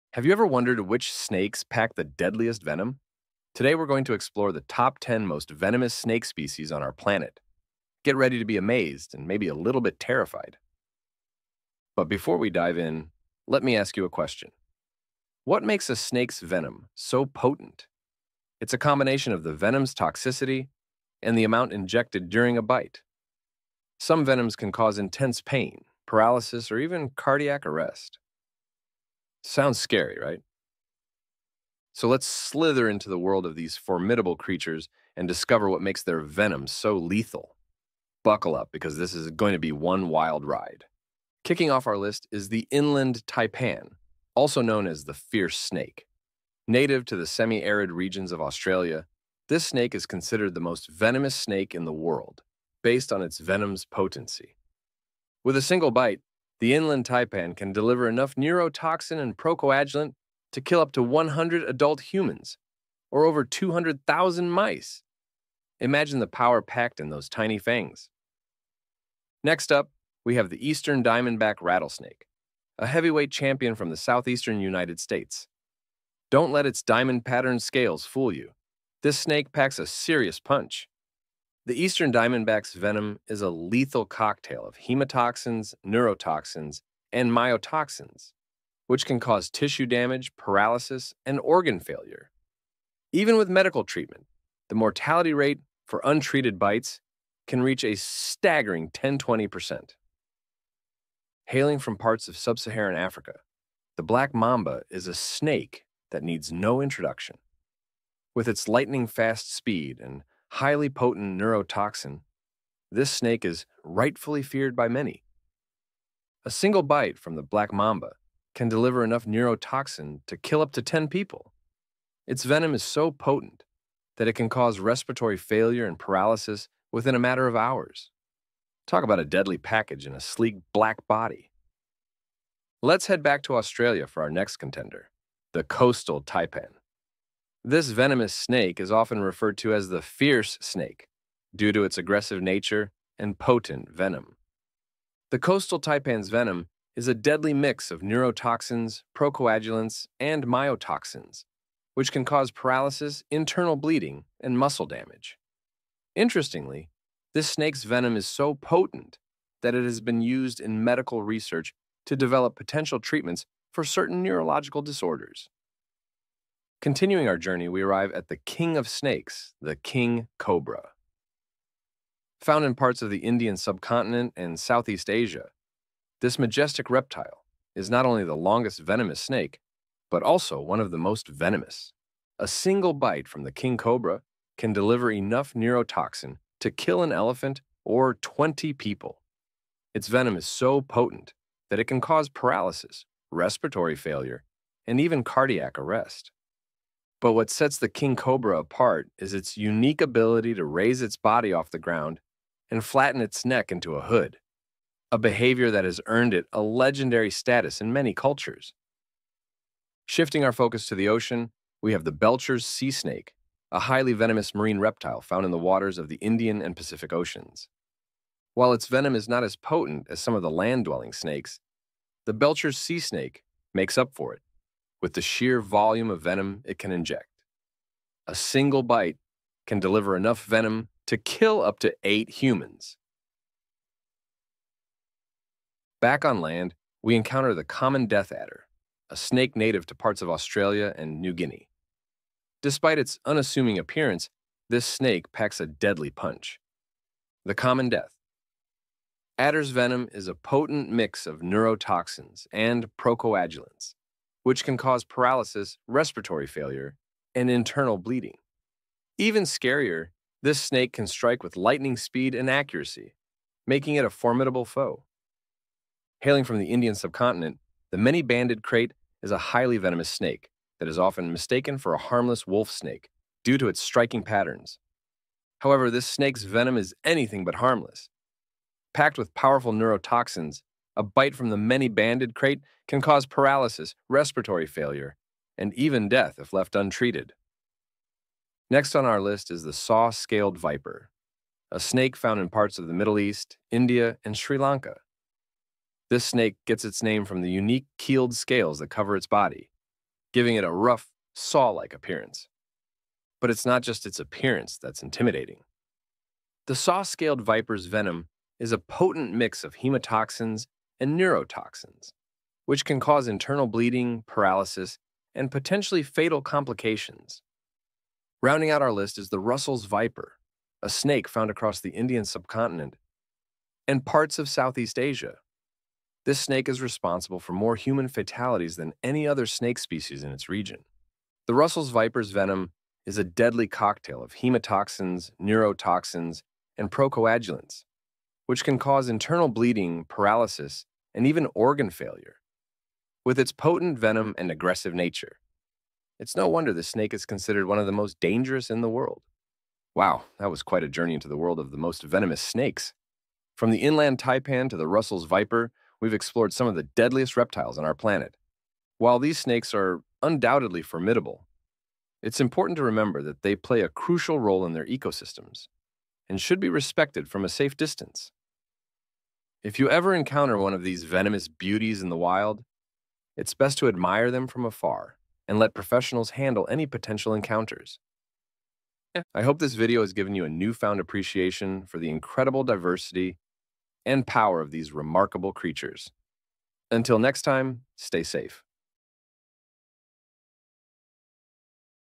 10 Deadliest Snakes: World's Most Venomous Reptiles Uncovered
Venomous snakes are some of the most feared creatures on the planet, and with good reason. Join us as we uncover the 10 deadliest snake species with the world's most potent venoms. From the lightning-fast Black Mamba to the mighty King Cobra, this video will take you on a thrilling journey through the serpentine world of these formidable predators.

Discover fascinating facts about each snake's venom composition, bite symptoms, and geographic distribution. Learn lifesaving tips on what to do in case of a venomous snakebite.